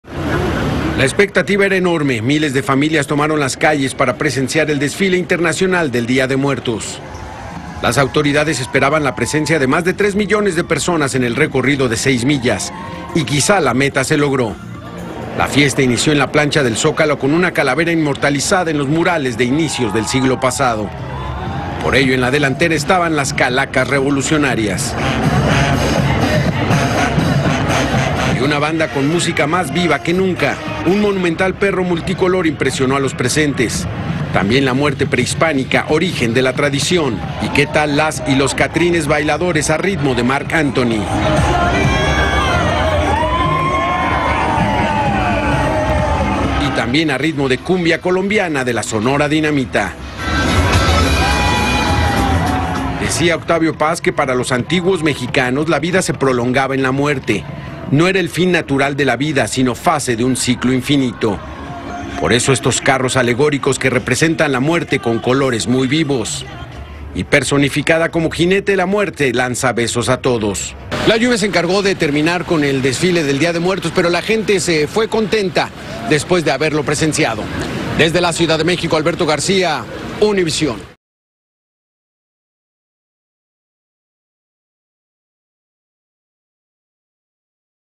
Los difuntos se pasearon por la Ciudad de México en los festejos de Día de Muertos
Desde El Zócalo, corazón de la capital mexicana, miles de personas disfrazadas desfilaron junto a enormes carrozas y figuras alegóricas. Personajes como Frida Kahlo, Cantinflas y Juan Gabriel cobraron vida durante este evento multitudinario.